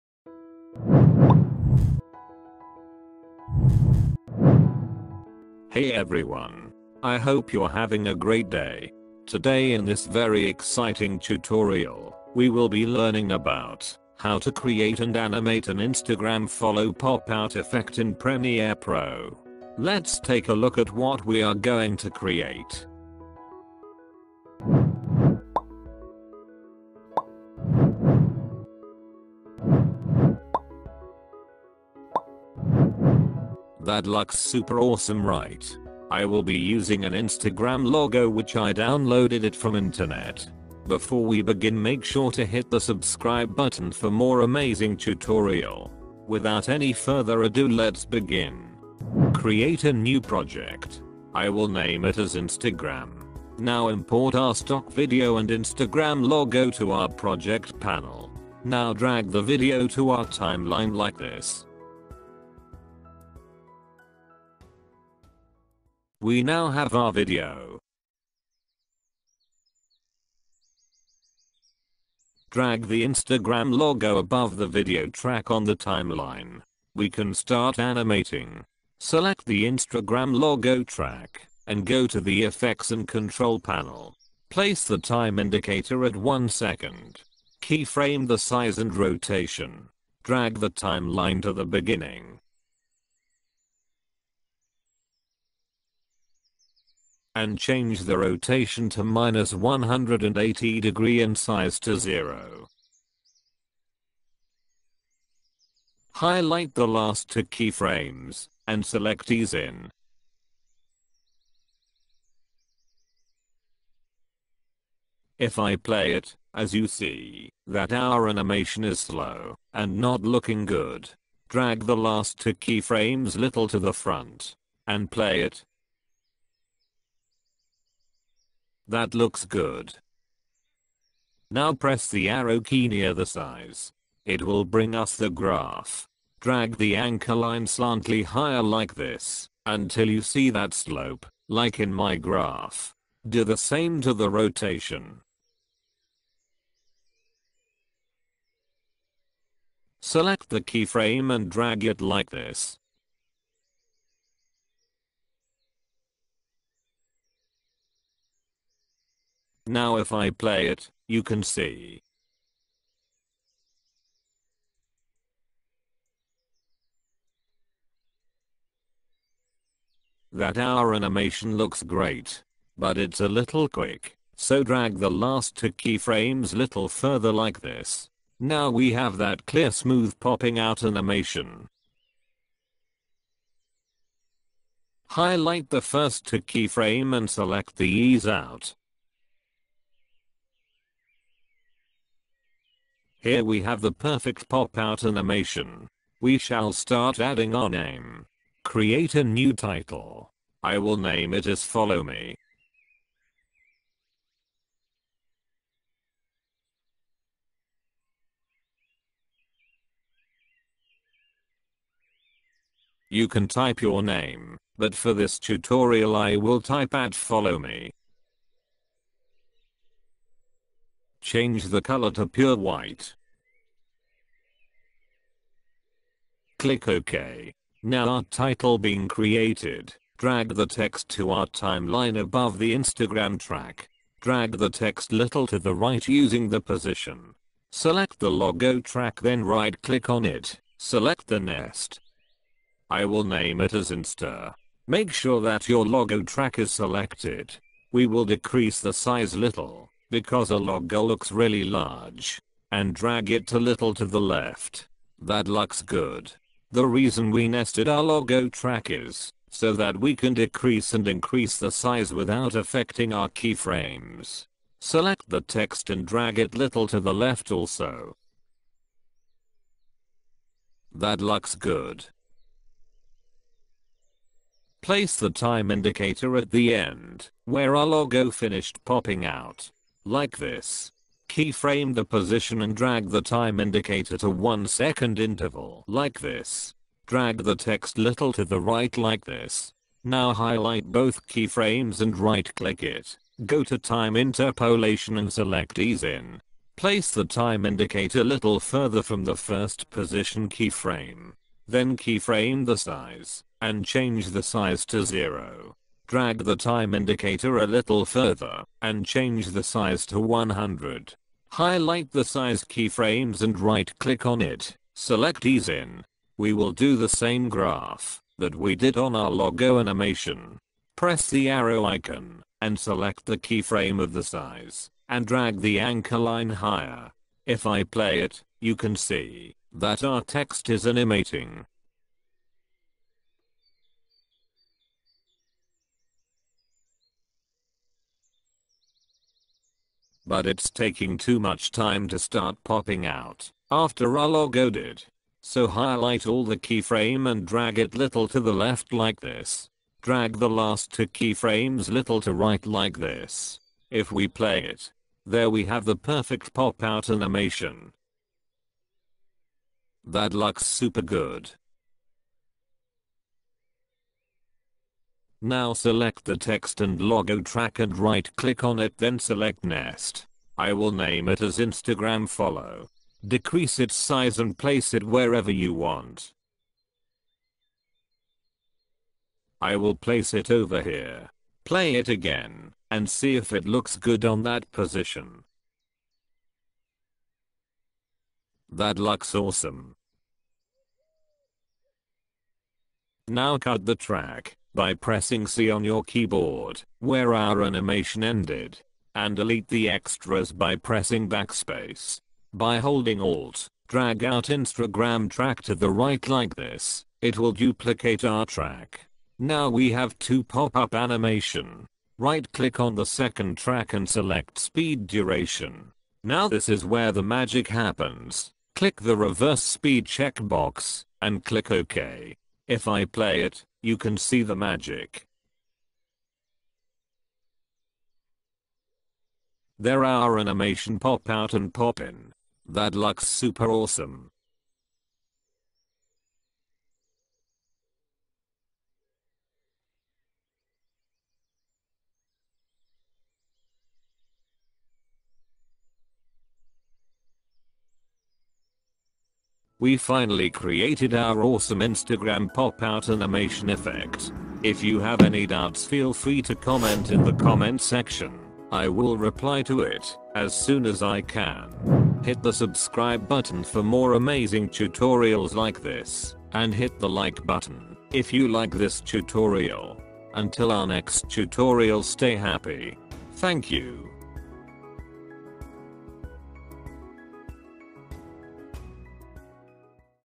Create Instagram "Follow me" Pop up animation in After Effects
If you like this video...click the like button.